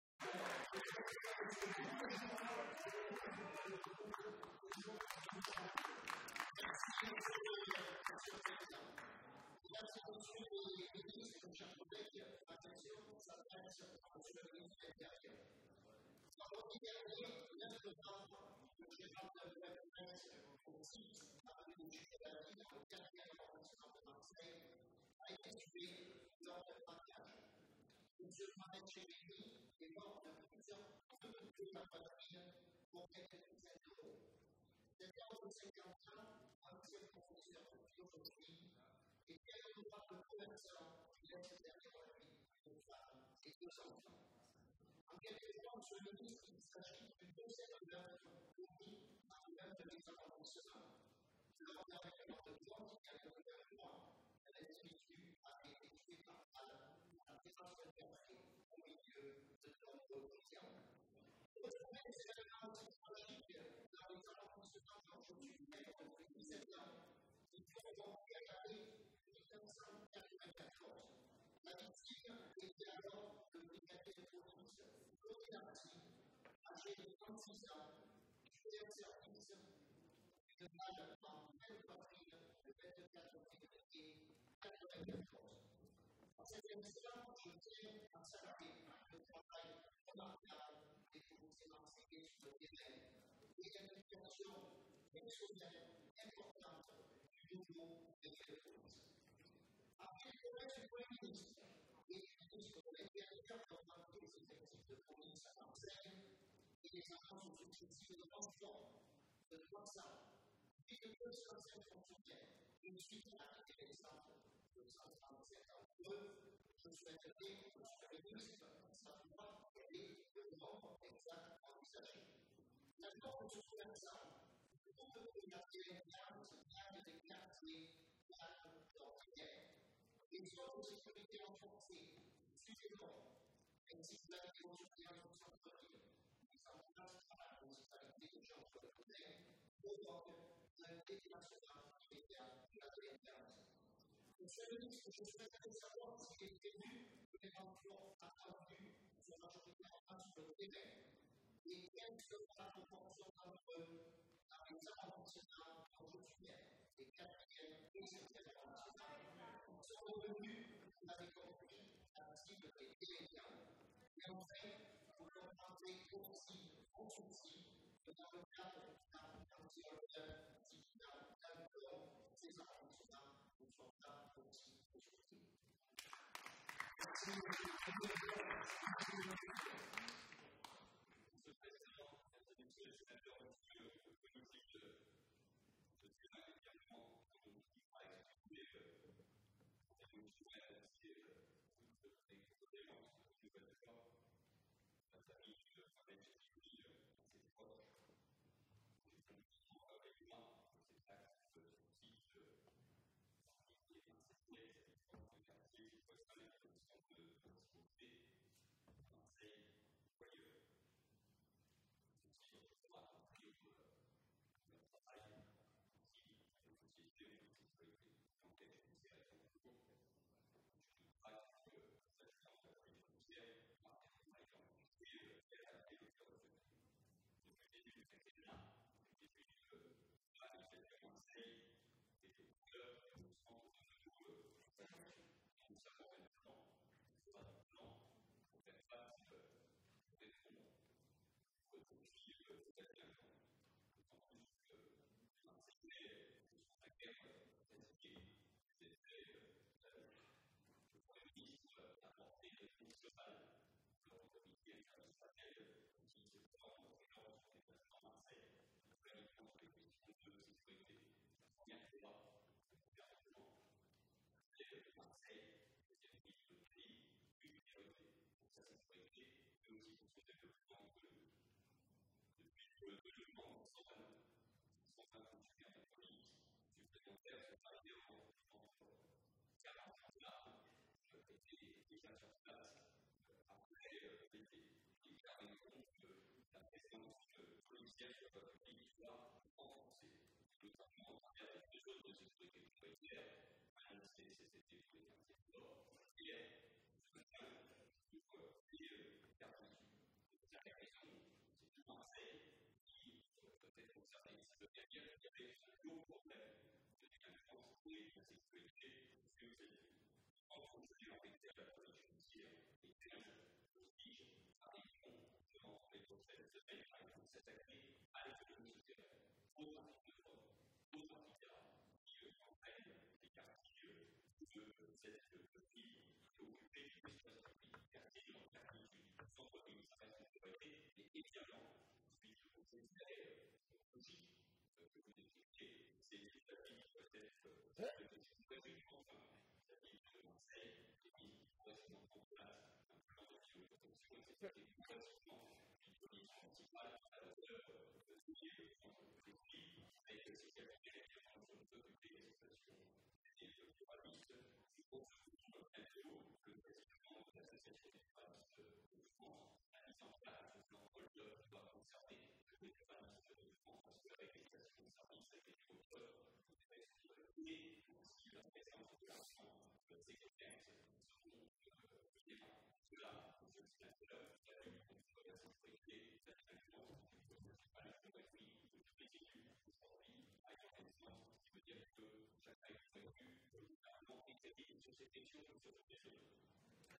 Bruno Gilles : Violences à Marseille et plan anti hold-up
Les actes de violences à Marseille se multiplient depuis plusieurs mois. Il faut remonter à 1994, rappelle Bruno Gilles (Bouches-du-Rône), pour retrouver pareille violence dans certains quartiers. L'arrivée de renforts annoncée par le Premier ministre et le ministre de l'intérieur est donc, dans ces conditions, une bonne nouvelle.

Toutefois, le sénateur s'inquiète du déplacement de la criminalité dans les quartiers calmes d'ordinaire, provoqué par la mise en place des zones de sécurité prioritaires (ZSP) nord et sud ainsi que la vidéosurveillance en centre-ville. Il souhaite donc connaitre les mesure du plan anti hold-up concernant zones d'ordinaire épargnées.